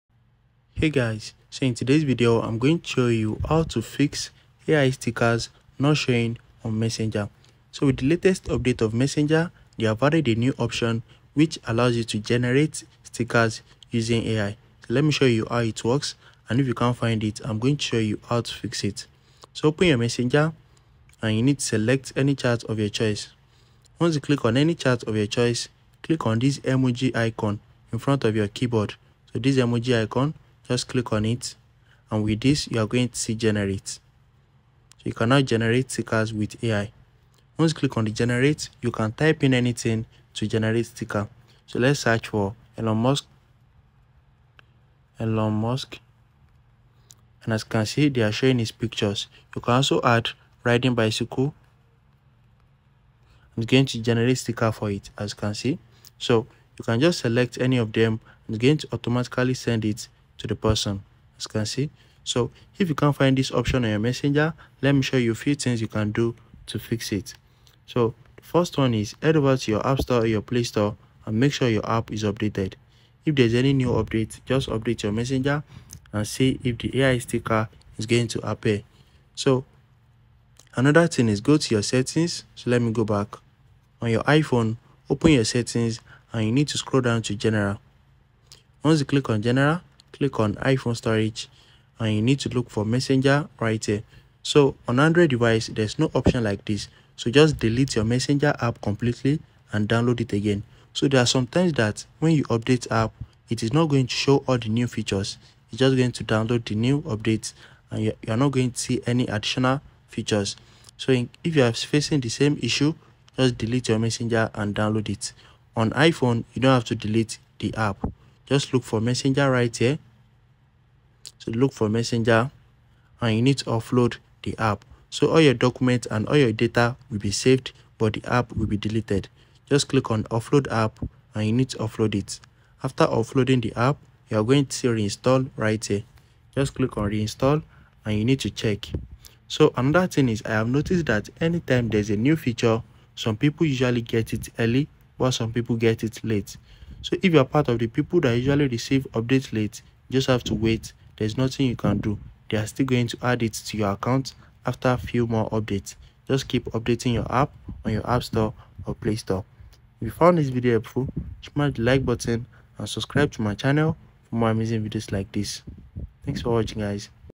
How to Fix AI Stickers Not Showing on Messenger
Discover how to fix AI stickers not showing on Messenger with our step-by-step troubleshooting guide! Whether you're encountering issues with accessing or using AI-generated stickers, we've got you covered. Watch now to learn effective solutions to ensure that you can enjoy the full range of dynamic stickers in your Messenger chats hassle-free!